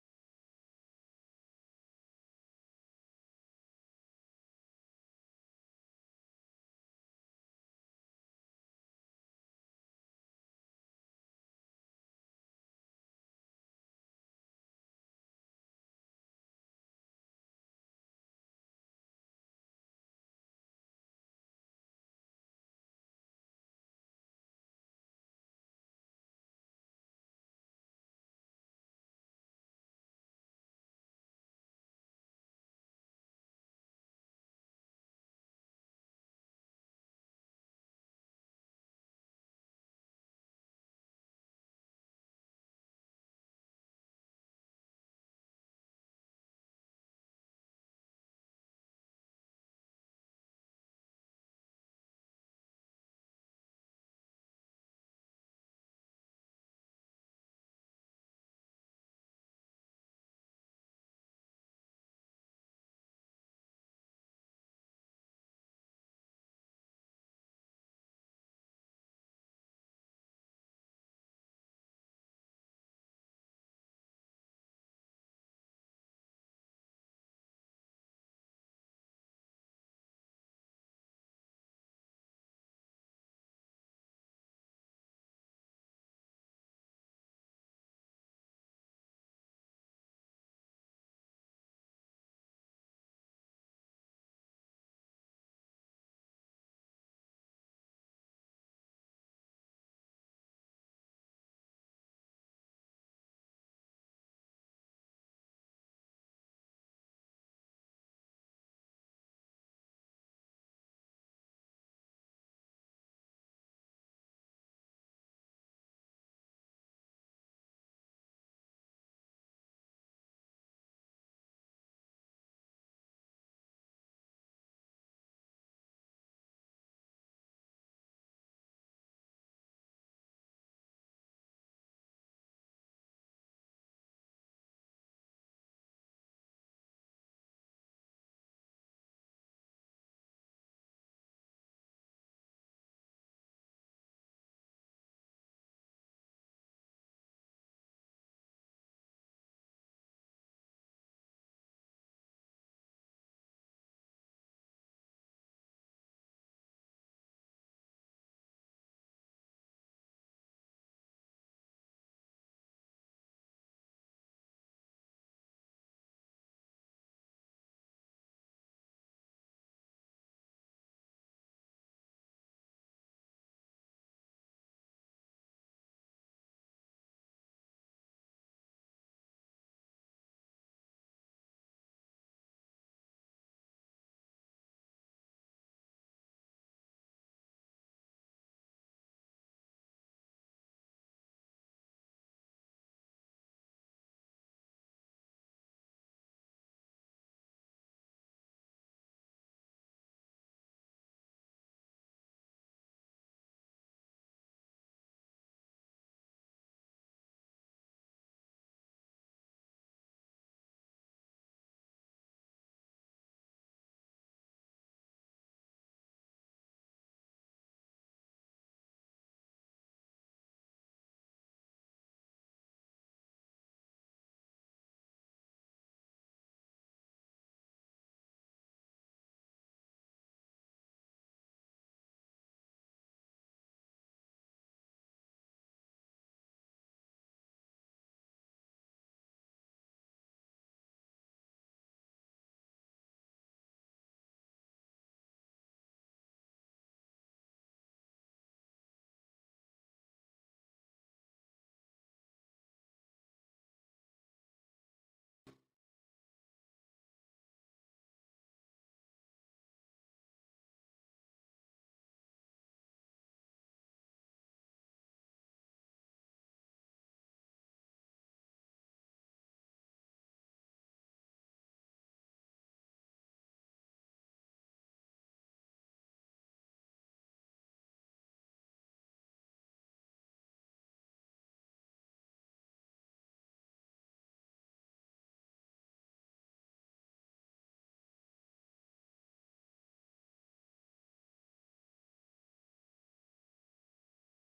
tearing down a shed with my kubota short version
time lapse of removing a shed so I can rebuild it at my house